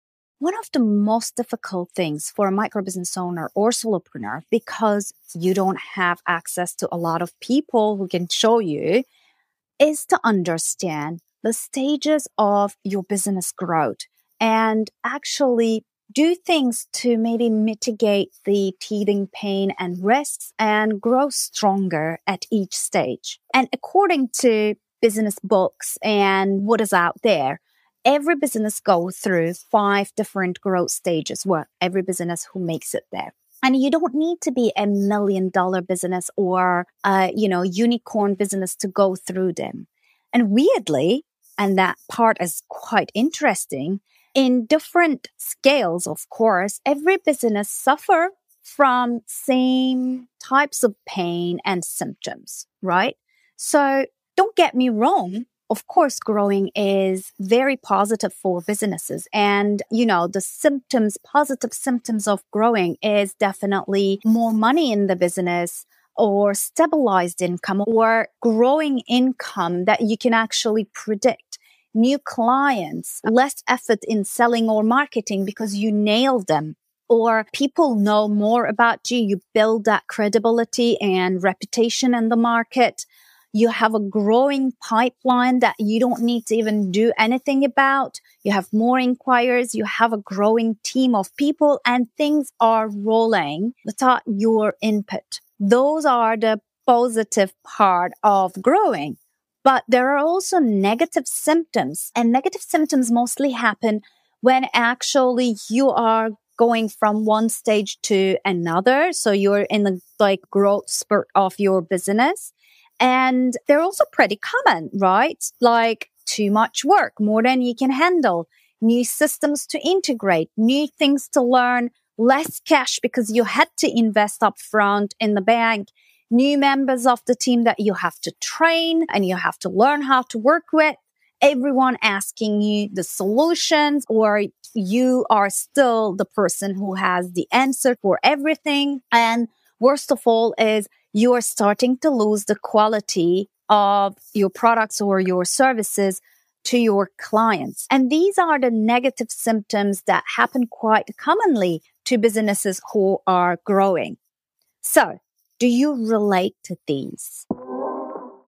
5 Stages of Business Growth and the key success factors
She is Awesome podcast episode 21- audiogramme
Developed and released by Ceylan boyce and Academy for Women Entrepreneurs